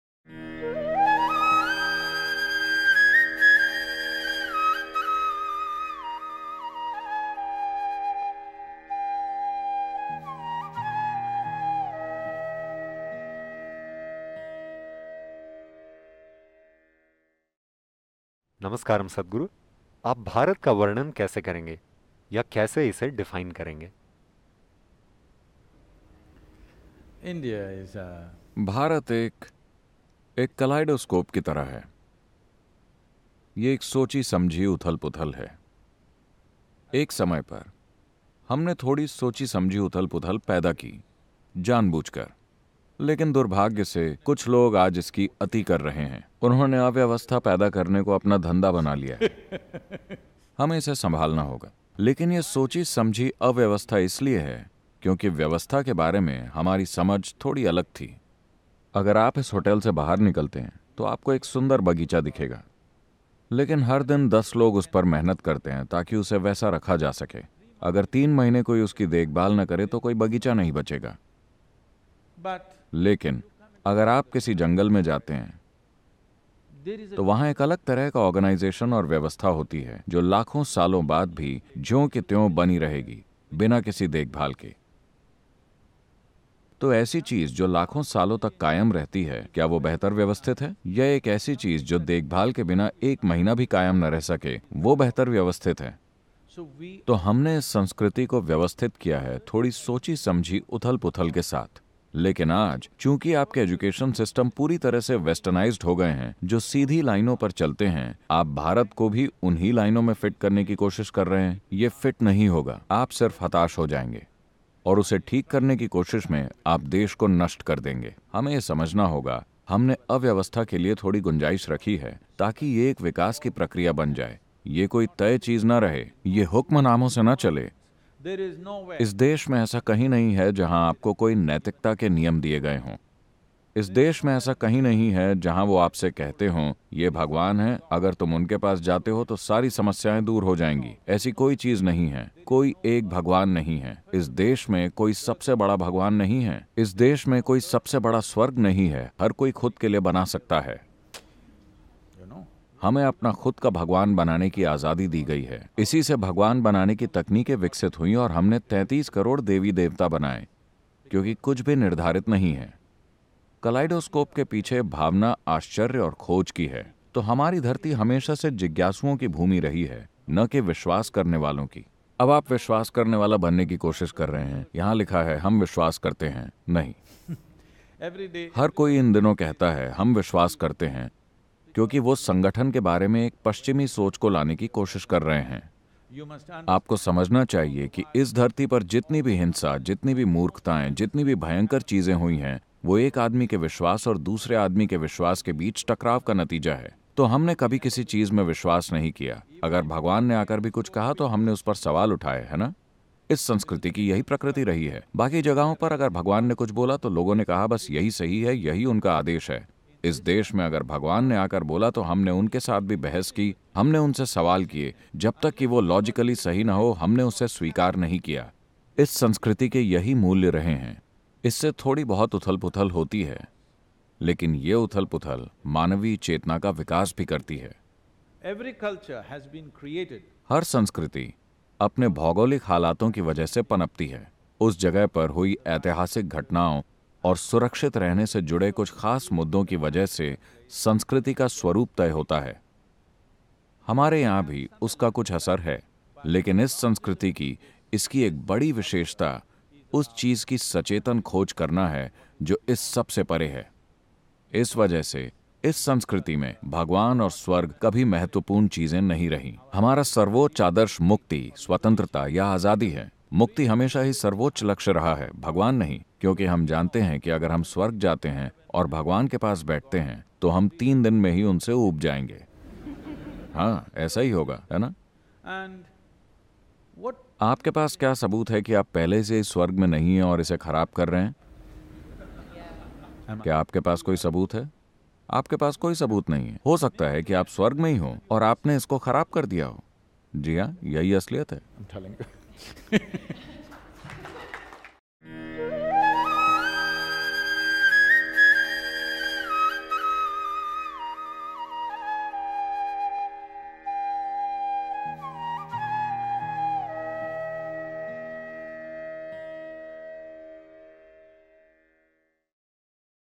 आप भारत को कैसे परिभाषित करेंगे? Aap bharat ko kaise paribhashit karenge.
जूही चावला ने सद्‌गुरु से पूछा कि ज़िन्दगी जीने का सबसे बढ़िया तारीख़ क्या है? हम क्या करें कि हम उस दिशा में बढ़ने लगें जहाँ हमें आनंद मिलेगा, ख़ुशी मिलेगी, जागरूकता मिलेगी और हम एक अच्छा सचेत जीवन जीने लगेंगे?